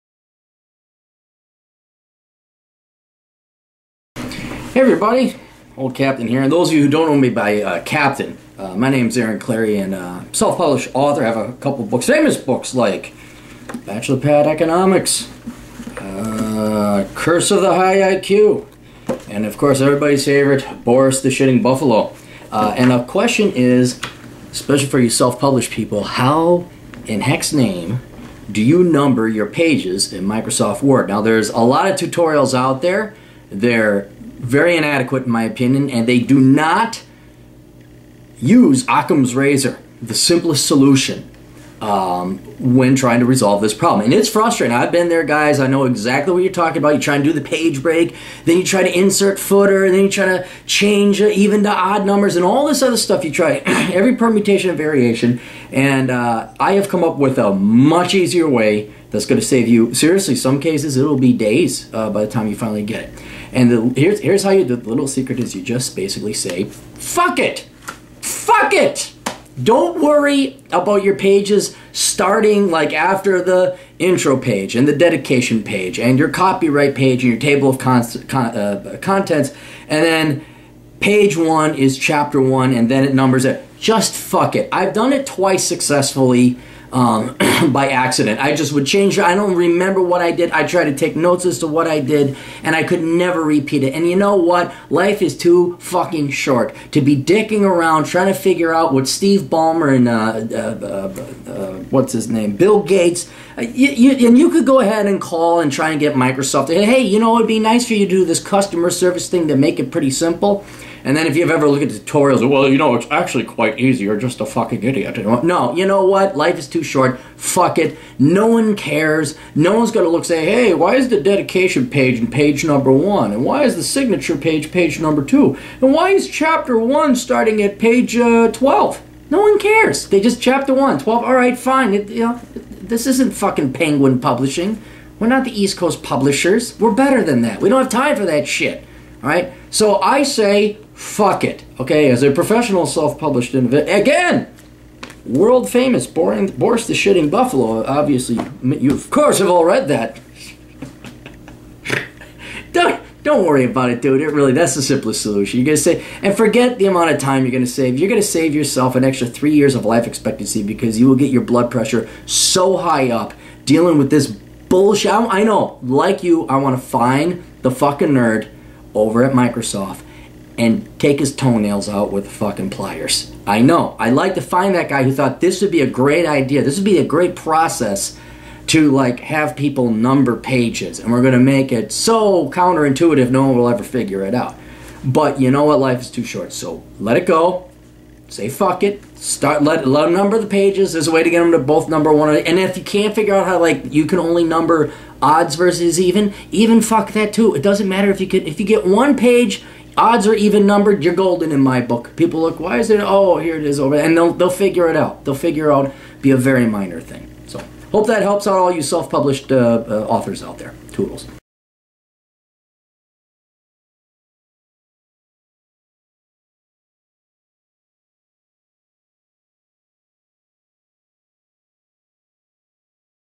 How to Get MS Word to Number Pages
Aaron Clarey, a self published author, shares his super easy secret to get MS Word to successfully number pages starting at "Page 1" "Chapter 1"